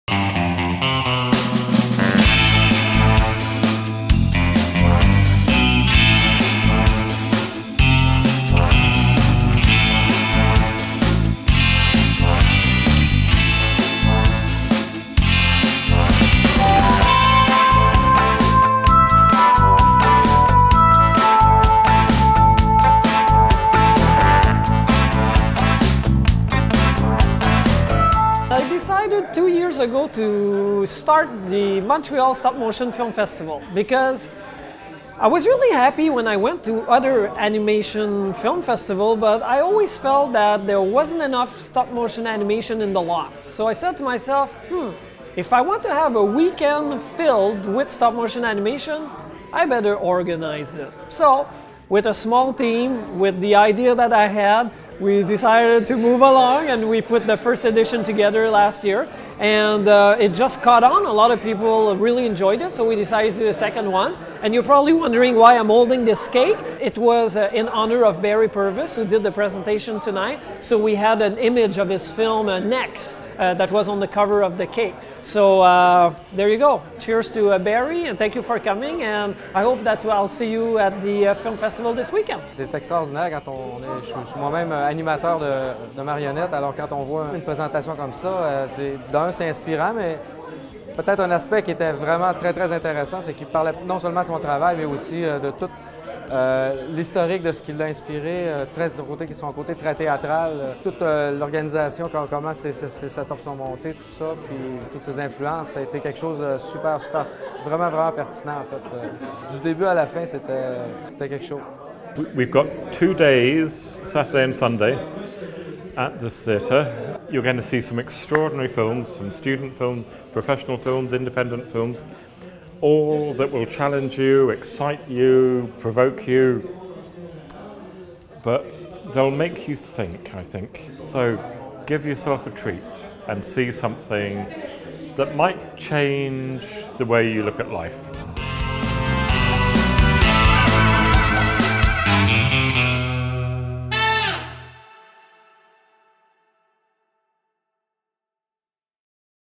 Montreal Stop Motion Film Festival - 2010 Overview
The 2010 Montreal Stop Motion Film Festival started off with a bang!  Erik Goulet created a truly a wonderful experience specifically geared to stop motion animation.  As a stop motion animator myself I can genuinely say this festival has the right sort of hands on feel that you get from great stop motion work films themselves.

Barry Purves opened the festival by delivering a three and a half hour tour de force stop motion Master Class revealing the techniques and inspiration behind many of his masterpieces.  It was a riveting presentation and even as Barry was bringing it to a close attenders called out for more.

After the Master Class everyone congregated around the cake table as Mr. Purves cut the celebratory cake.  Erik Goulet was a gracious festival director throughout the event and made a point of remembering everyone's first name upon meeting them as well as making sure they had as much cake as they liked.

All in all it was a fantastic opening to a wonderful event and even though this was my first time meeting everyone involved I felt instantly at home.  I can't wait for a chance to go back again.

-Grayden Laing.

This video was produced by Grayden Laing for Canadian Animation